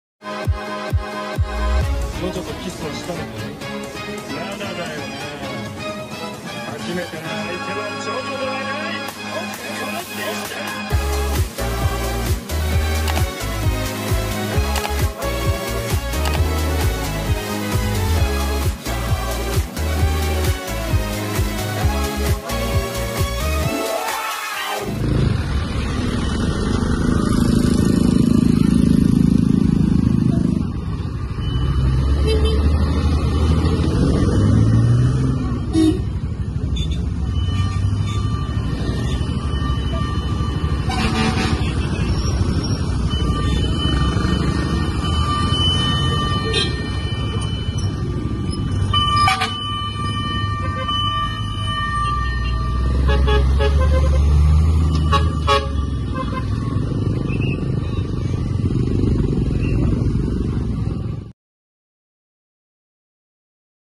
(10-70) Positive Alarm - Firetrucks Responded to Clariven St, Tondo, Manila
Hello, i am zitow a Emergency Vehicles Enthusiast. I make content about Emergency Vehicles, Bus, and Trains.

That's all for today's video,